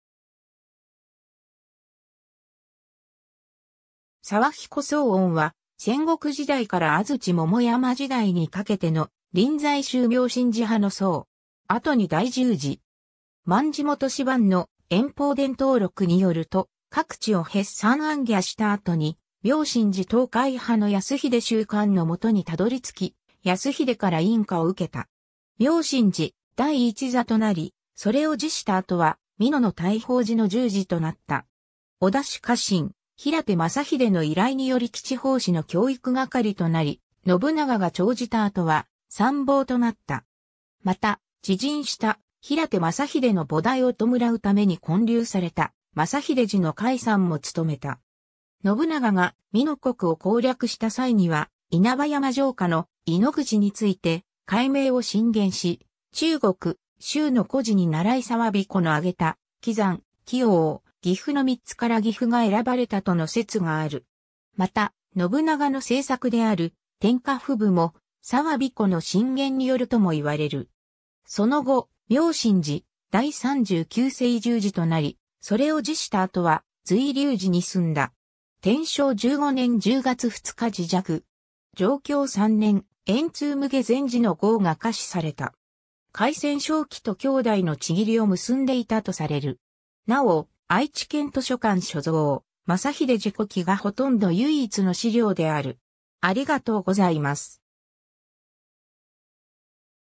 沢彦
日本の禅僧_(臨済宗)
室町・安土桃山時代の僧
沢彦宗恩（たくげんそうおん、生年不詳 - 天正15年10月2日（1587年11月2日））は、戦国時代から安土桃山時代にかけての臨済宗妙心寺派の僧。
後に大住持。
 卍元師蛮の『延宝伝灯録』によると、各地を歴参行脚した後に妙心寺東海派の泰秀宗韓の元に辿り着き、泰秀から印可を受けた。
妙心寺第一座となり、それを辞した後は美濃の大宝寺の住持となった。
 織田氏家臣・平手政秀の依頼により吉法師（後の織田信長）の教育係となり、信長が長じた後は参謀となった。
また、自刃した平手政秀の菩提を弔うために建立された政秀寺の開山も務めた。
信長が美濃国を攻略した際には稲葉山城下の「井ノ口」について改名を進言し、中国・周の故事にならい沢彦の挙げた「岐山・岐陽・岐阜」の3つから岐阜が選ばれたとの説がある。
また、信長の政策である天下布武も沢彦の進言によるともいわれる。
 その後、妙心寺第39世住持となり、それを辞した後は瑞龍寺に住んだ。
天正15年（1587年）10月2日示寂。
貞享3年（1686年）、「円通無礙禅師」の号が下賜された。
 快川紹喜と兄弟の契りを結んでいたとされる。
 なお、愛知県図書館所蔵『政秀寺古記』がほとんど唯一の資料である。